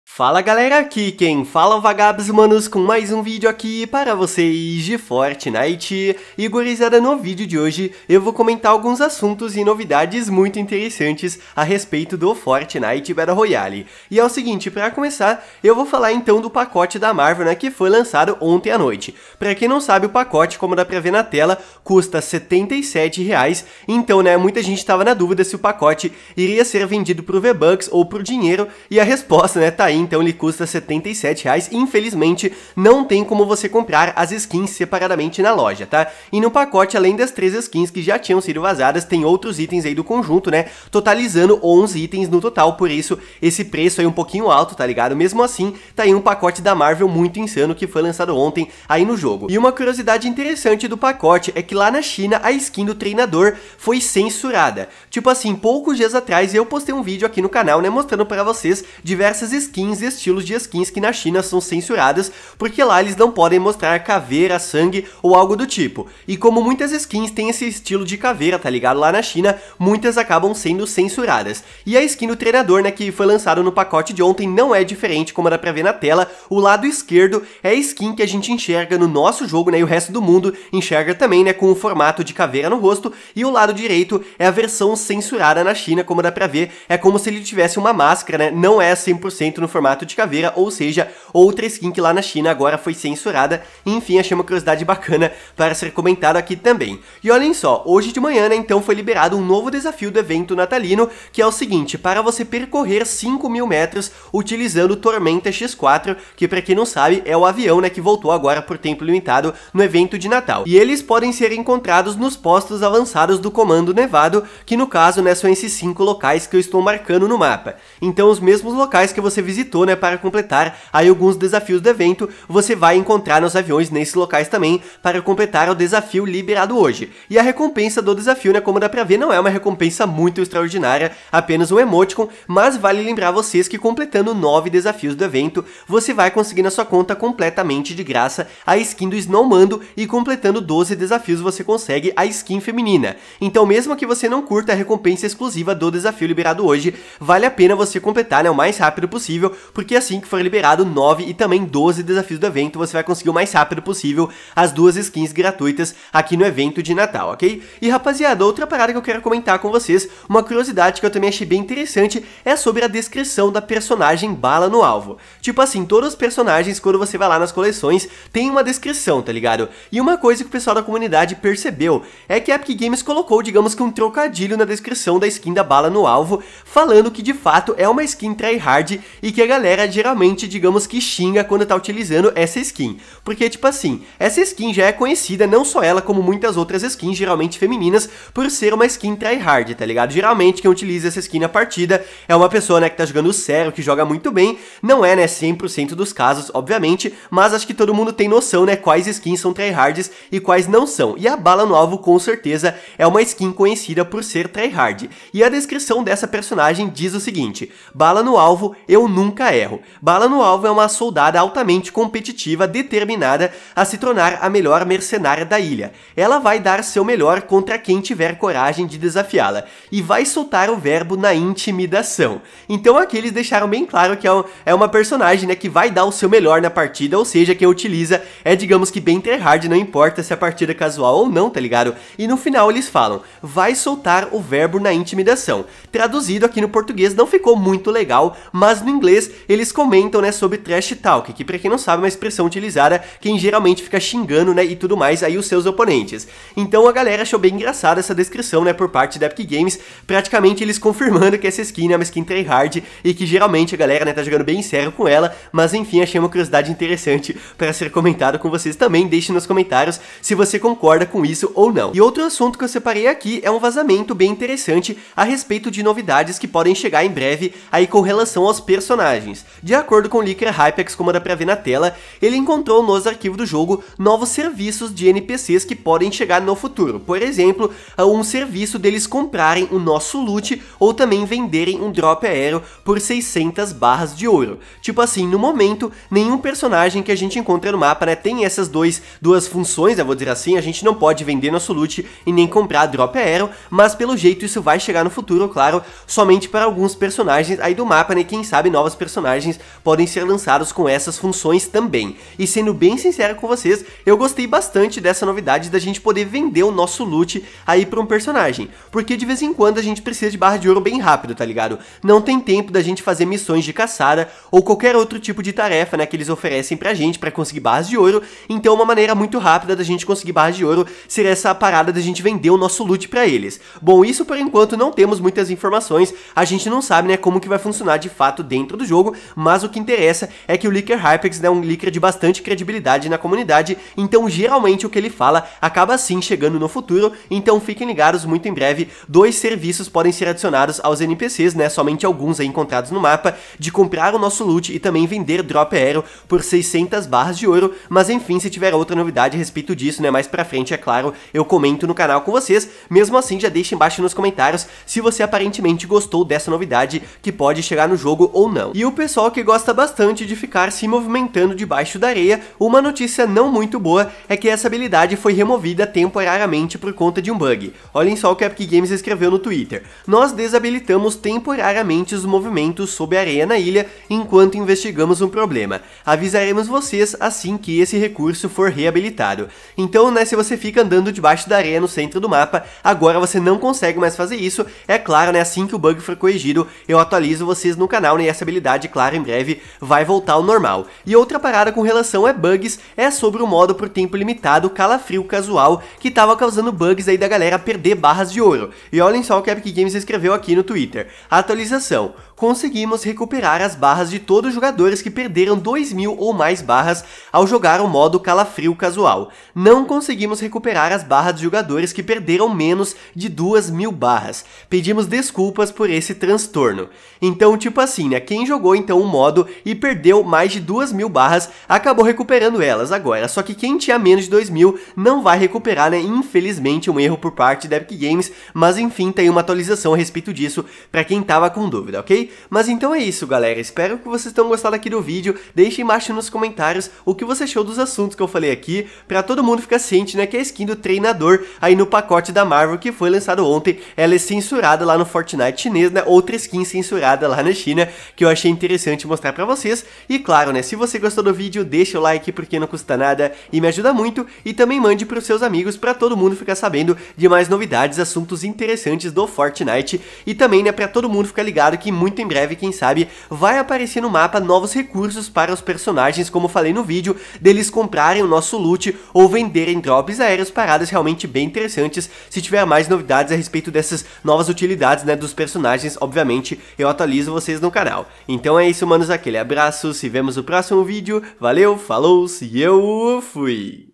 FORTNITE - SKIN TRYHARD, NOVA CENSURA e VENDER LOOT?
Fala gurizada, nesse vídeo comento novidades do Fortnite Battle Royale, skin Tryhard, novas habilidades personagens NPC, skin censurada e mais. Espero que gostem :)